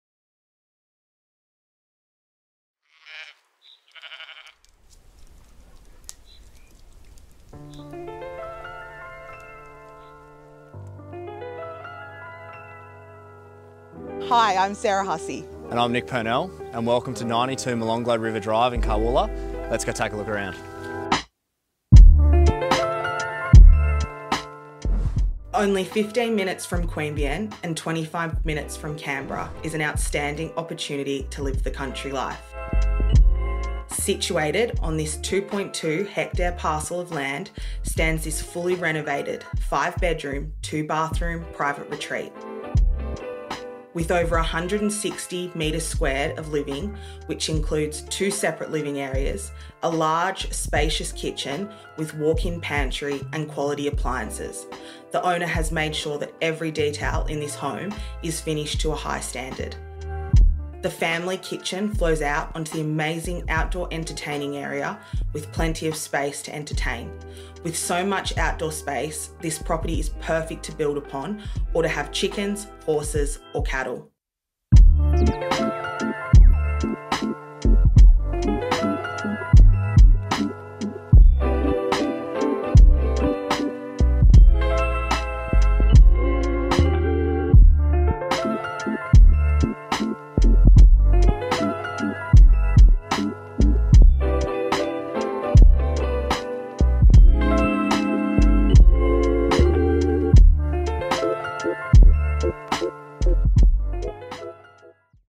Purnell Real Estate - 92 Molonglo River Drive Carwoola
Rural Retreat.

92 Molonglo River Drive, Carwoola has been fully renovated by reputable, family owned construction company, this means all the hard work has been done to set this property up and is now ready for you to reap the rewards. This property is sure to impress, located only 15 minutes from Queanbeyan and 25 minutes from Canberra this is an outstanding opportunity to live the country life.

Situated on this 2.2 hectare parcel of land stands this fully renovated five bedroom, two bathroom private retreat.

With over 160m2 of living which includes two separate living areas, large spacious kitchen with walk in pantry and quality appliances, the owner has made sure that every detail in this home is finished to a high standard.

The family kitchen flows out onto the amazing outdoor entertaining area with plenty of space to entertain, with so much outdoor space this property is perfect to build upon or to have chickens, horses or cattle. The block is perfect for grazing and has a huge dam situated towards the back of the property while the bore that has been drilled and is ready for a pump to be installed.

Four of the five spacious bedrooms are serviced by a large family bathroom with a bathtub while the main bedroom has its own walk in wardrobe and ensuite.

The property has a wood fire heater and split system for year round comfort.

The owner has left no stone unturned installing a new sewerage system and fencing around the property as well as a shed located close to the home and is 12x7 which can easily fit four cars.

This is a property you do not want to miss out on.